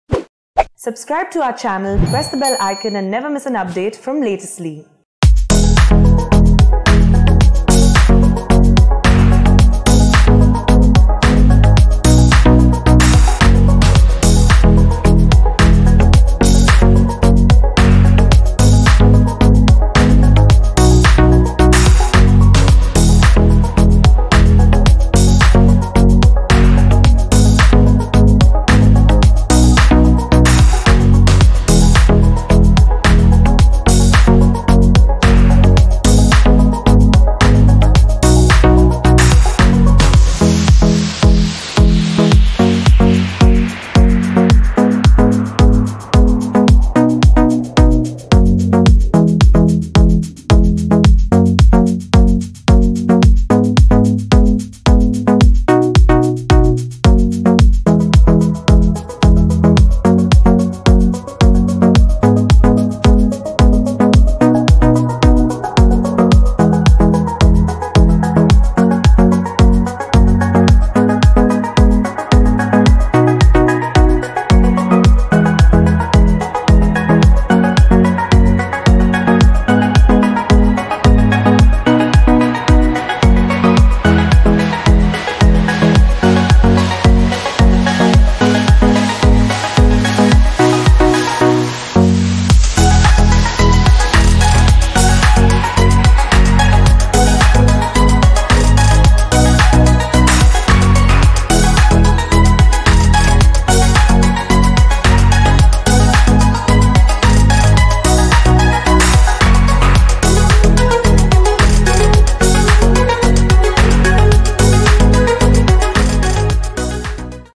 Fauji 2: Shah Rukh Khan's Iconic 1989 Show Gets Sequel With Vikas Jain & Gauahar Khan In Lead Roles
Shah Rukh Khan's 1989 television series 'Fauji' is set for a modern revival. The updated series will feature Gauahar Khan and Vicky Jain in the lead roles. Vikas Jain, Ankita Lokhande's husband, will make his mainstream television debut as Colonel Sanjay Singh in 'Fauji 2', whereas Gauahar Khan will portray Lieutenant Colonel Simarjeet Kaur in the show. Watch video!